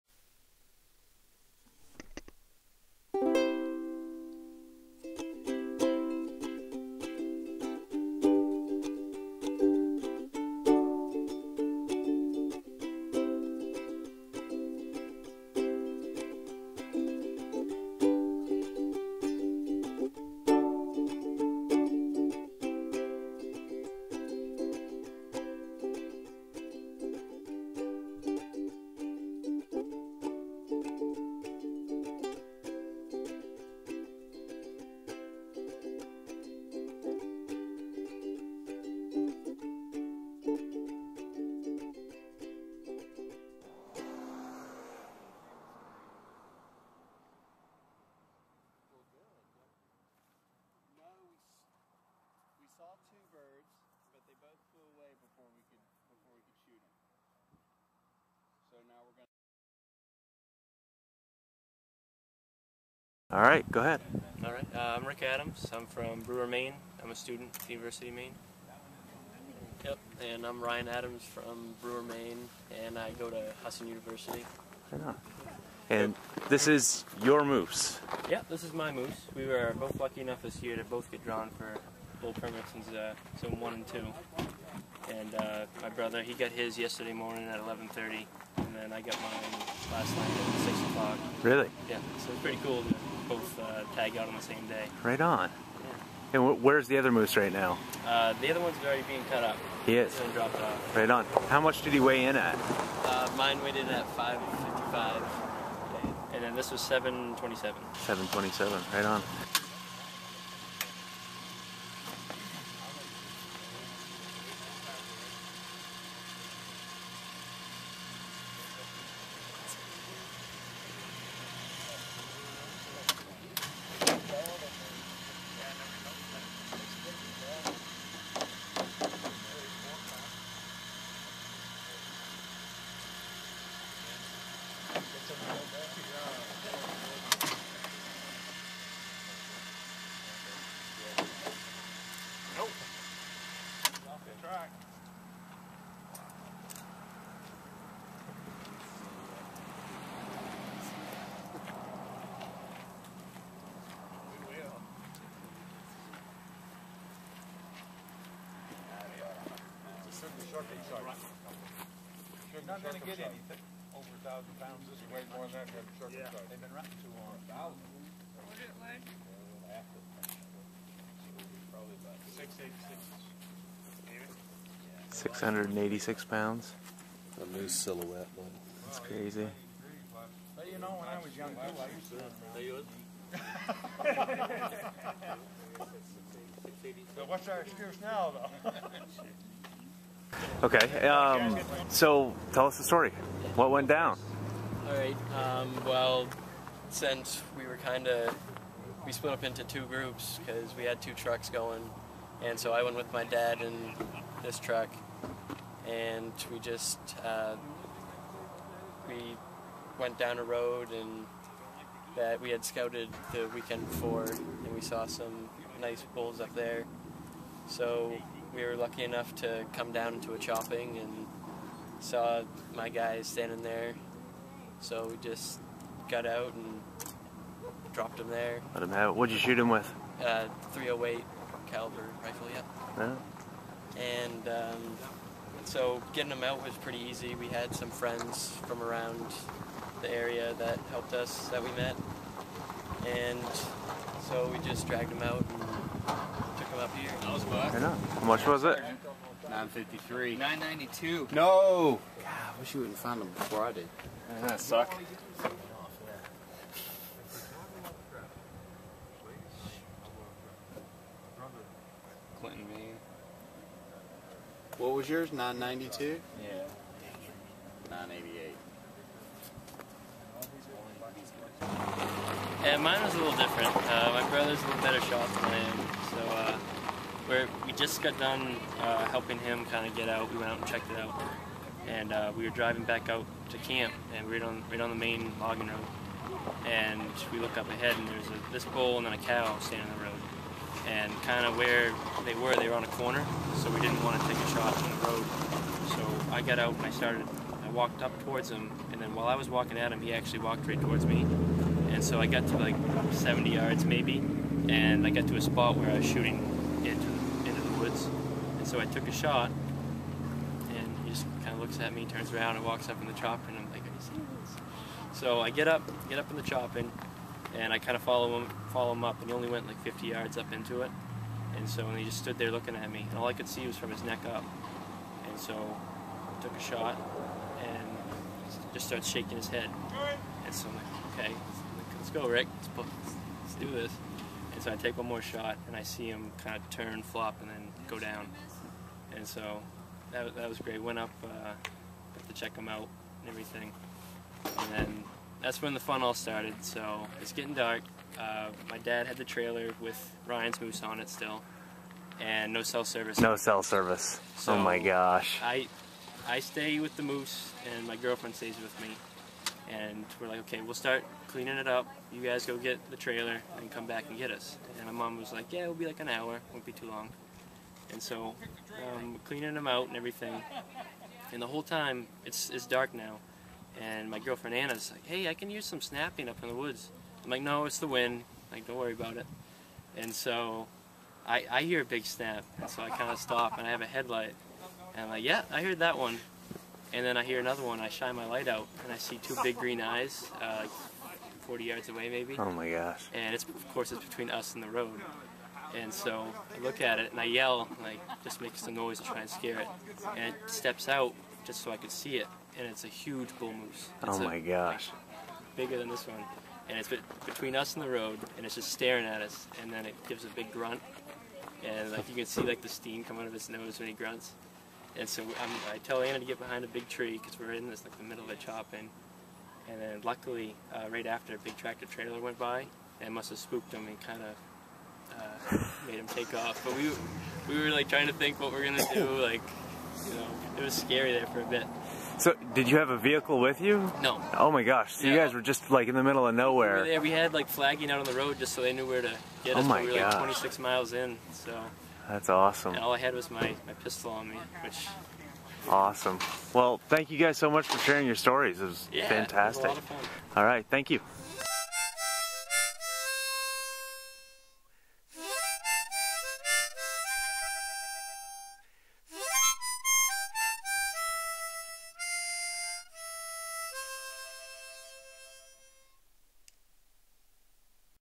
The Sharpened Axe Hits the Road - Part 4.wmv
Rick and Ryan Adams from Brewer, Maine talk about hunting moose in the North Maine Woods.